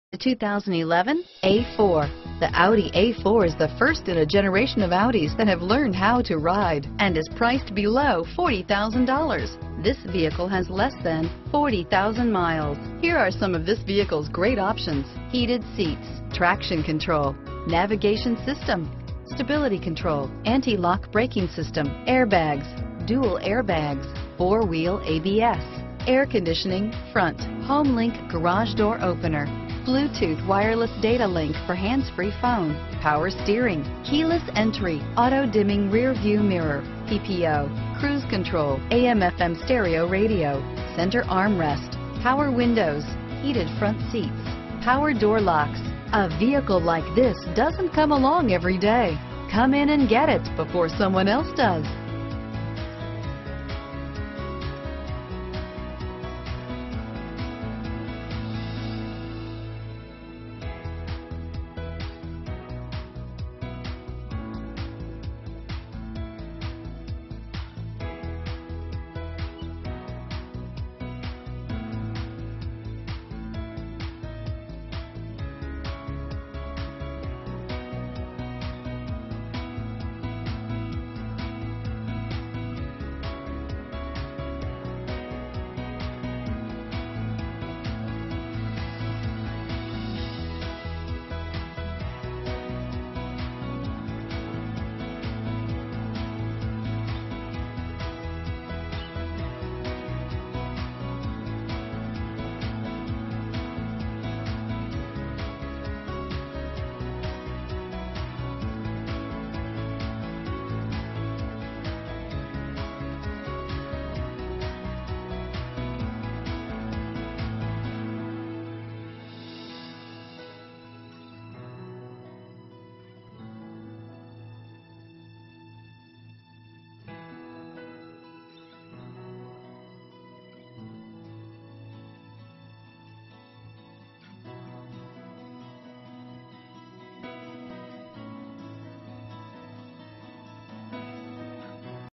2011 Audi A4 - 2.0T Avant Quattro Premium Wagon 4D Los Angeles Van Nuys Santa Monica Bever
2011 Audi A4

Keyes Audi
5239 Van Nuys Boulevard

font color = red size = 4TITANIUM SPORT PACKAGE, NAVIGATION!/font color text size This ONE OWNER Audi A4 Avant Wagon is Phantom Black Pearl Effect with Black Leather Interior. This CERTIFIED A4 Avant has NAVIGATION, Bang & Olufsen Premium Sound System, Titanium Sport Package with Sport Suspension, Sport Steering Wheel with Paddle Shifters, Sport Front Seats with Lumbar Support, Rearview Camera with Parking Sensors, Voice Control, Premium Plus Package with Xenon Headlights, LED Daytime Running Lights, Heated Seats, Bluetooth Wireless, iPod Interface, 3-Zone Climate Control, Sirius Satellite Radio with SD Card Slot, Sunroof, Multi-Spoke Alloy Wheels, and more. font color = blue size = 4Ask about 0.9% APR short term financing on approved credit!/font color text size. Keyes Audi is the #1 volume Audi dealer in the United States!